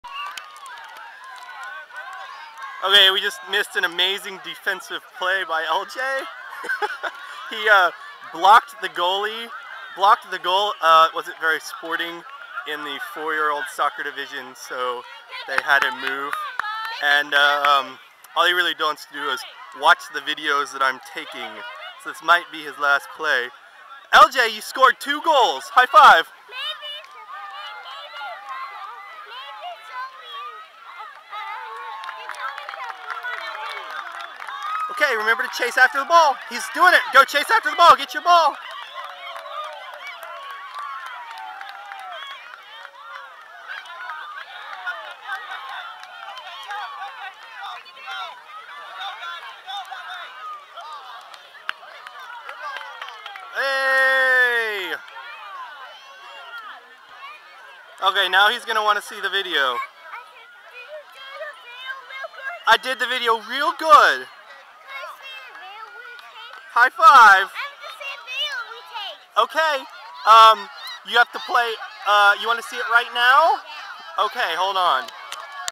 Third Goal
This was LJ's third goal, still the first half.  Since I was concentrating so much on trying to convince him to stay in the game after each play, I missed filming his spectacular second goal.  I explained to him earlier that the point of soccer was to kick the ball in your net while preventing the other team from kicking it in their net.  Unfortunately LJ took this advice literally and ran to the goal he was supposed to defend and blocked it with his entire body (before the ball was even close).  Obviously the other team couldn't score, but the coaches/refs weren't as amused as I was.  He was undettered and took the ball he blocked all the way across the field to score his second goal.